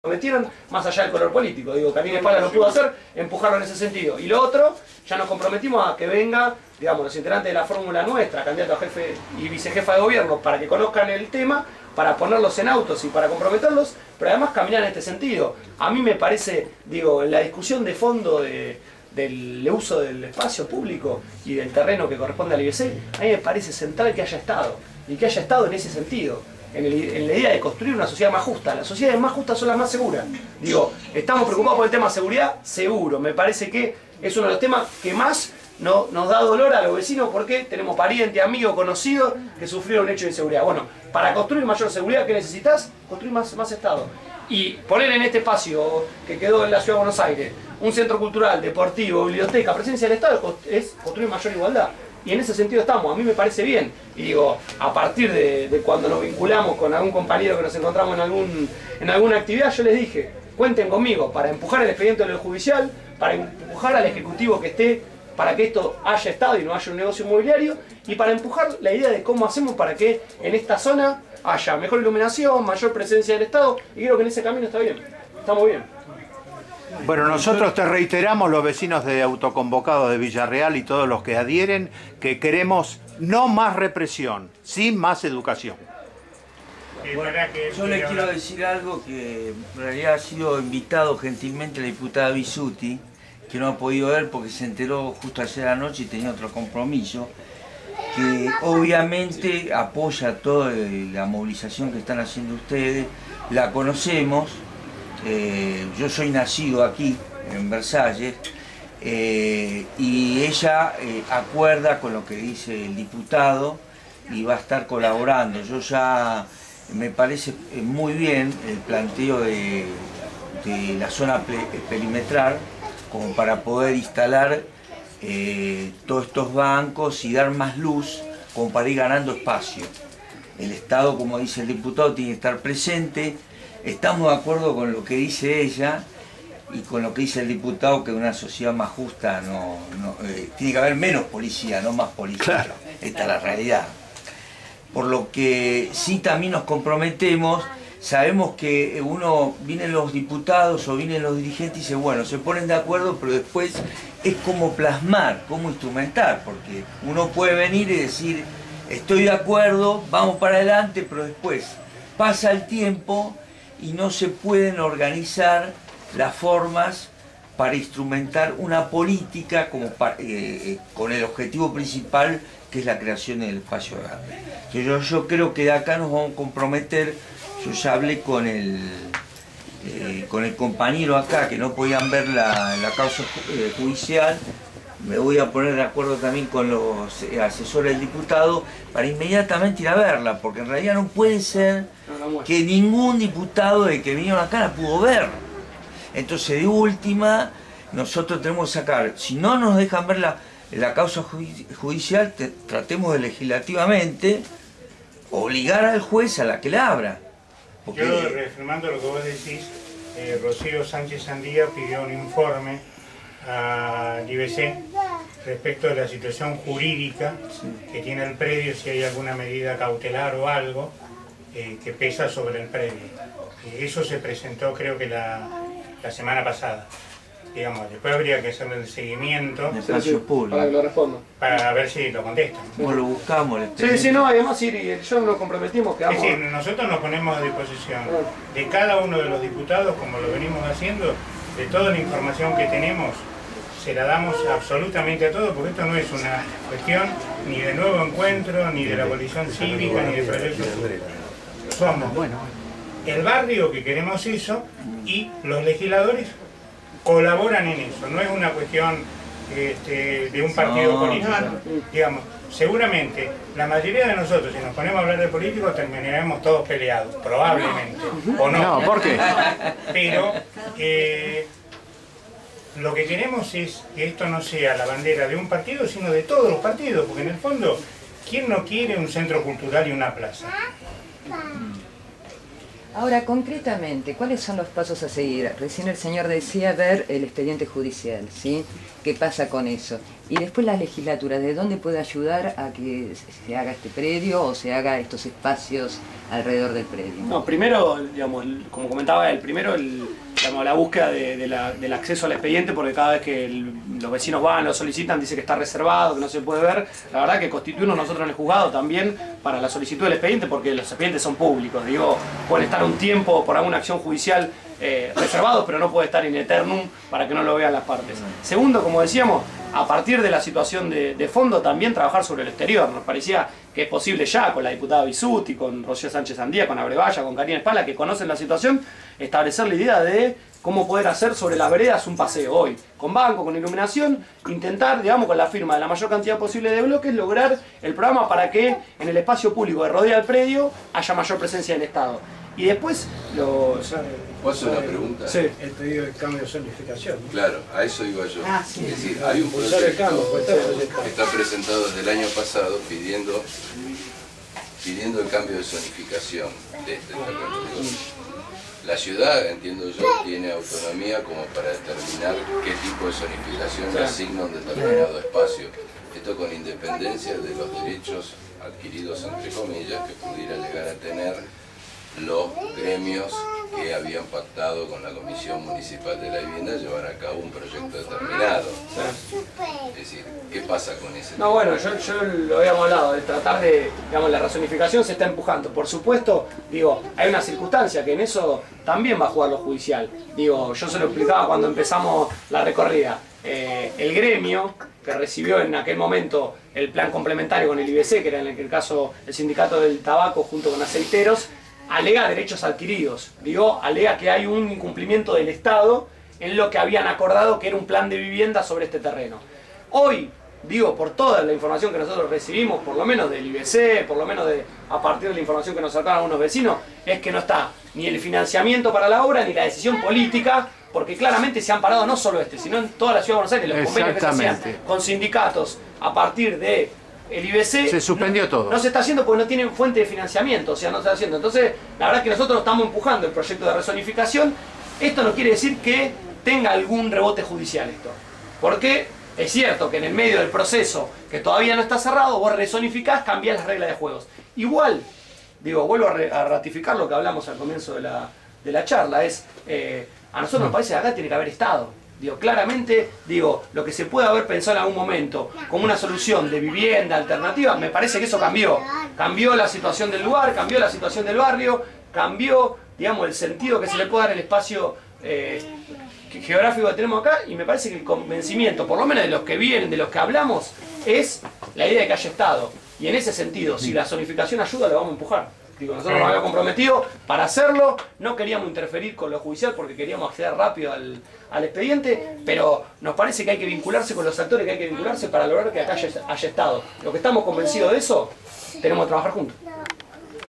Diputado Gonzalo Ruanova con Vecinos Autoconvocados de Villa Real 4/4
En la fecha se acercó a nuestro barrio, el Diputado por la CABA  Gonzalo Ruanova, del partido Nuevo Encuentro, y el Director de la Comisión de Desarrollo Económico Mercosur y Políticas de Empleo, Dr. Ruben F. Haber, en representación de la Legisladora Delia Bissutti,  a interiorizarse de los problemas que nos crea a los vecinos de Villa Real, el estado de abandono del predio ex Agrocom, delimitado por las calles: Moliere, Ramon Lista, Virgilio, y José P. Varela, del barrio de Villa Real de CABA,  por parte del Gobierno de la Ciudad Autónoma de Buenos Aires.
Conjuntamente al grupo de Vecinos Autoconvocados de Villa Real, (un grupo de unas 50 personas), recorrieron el perímetro y fueron invitados por los hermanos Spaggiari, a tener una visión aérea desde su azotea, del estado actual de abandono del citado predio, y a interiorizarse de las tratativas de los Vecinos para el logro de sus aspiraciones de tener un barrio mejor, y digno de ser vivido. Posteriormente, todos fuimos recibidos en el domocilio de la Dra. Elisa Raida de la calle J. P. Varela, (frentista del predio), con un lunch que ella ofreció a los asistentes.